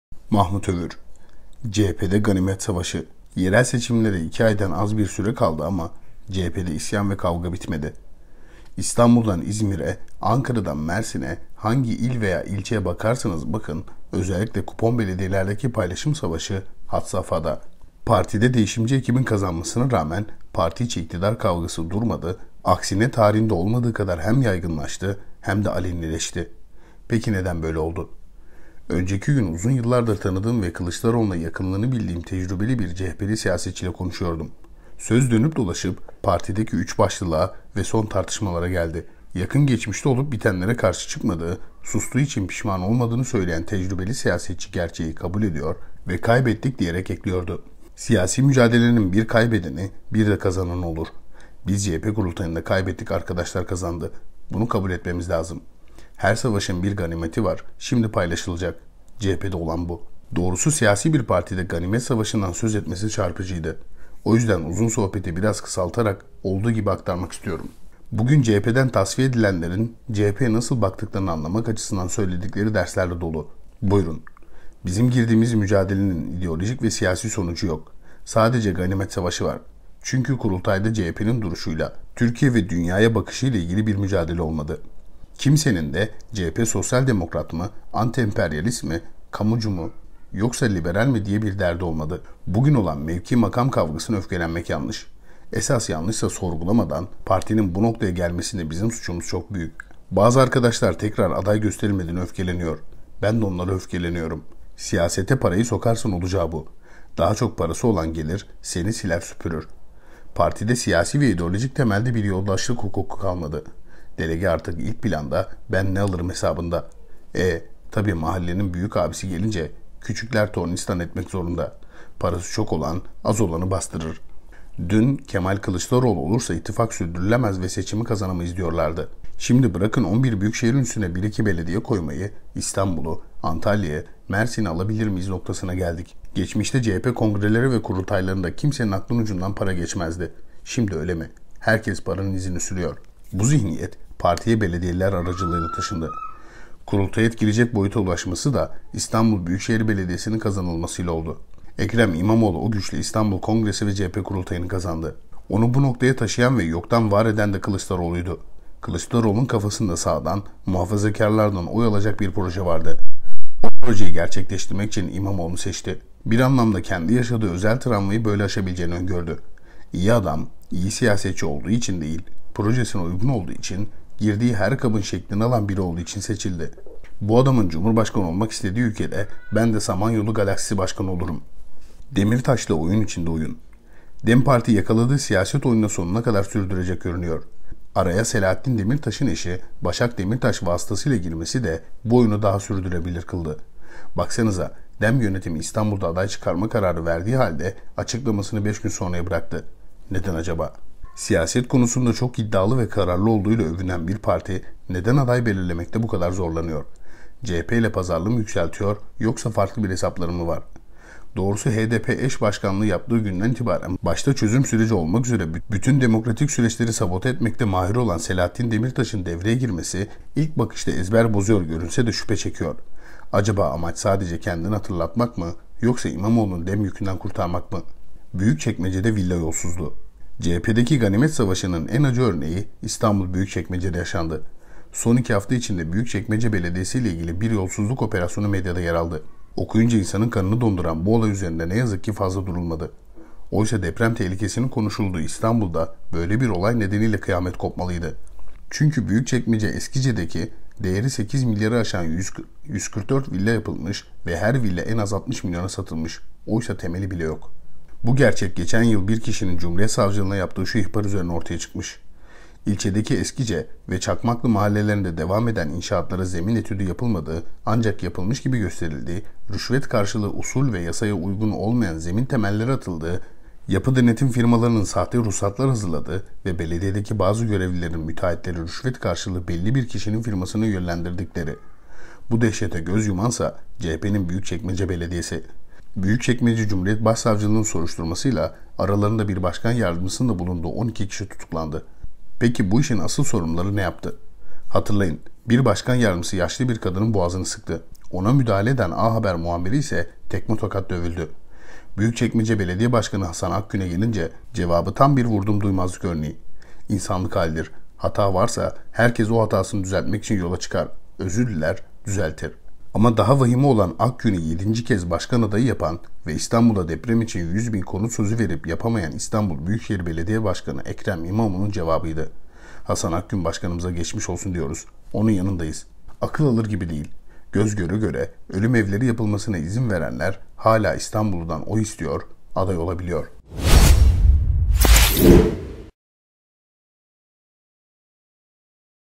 CHP'DE 'GANİMET' SAVAŞI!
Sabah gazetesi yazarı Mahmut Övür köşe yazısında ''Yerel seçimlere iki aydan az bir süre kaldı ama CHP'de isyan ve kavga bitmedi. İstanbul'dan İzmir'e Ankara'dan Mersin'e hangi il veya ilçeye bakarsanız bakın özellikle "kupon" belediyelerdeki paylaşım savaşı had safhada. Partide "değişimci" ekibin kazanmasına rağmen parti içi iktidar kavgası durmadı tam aksine tarihinde olmadığı kadar hem yaygınlaştı hem de alenileşti. Peki neden böyle oldu?Önceki gün uzun yıllardır tanıdığım ve Kılıçdaroğlu'na yakınlığını bildiğim tecrübeli bir CHP'li siyasetçiyle konuşuyordum. Söz dönüp dolaşıp partideki "üç başlılığa" ve son tartışmalara geldi'' dedi.

Merhaba,
Hergün...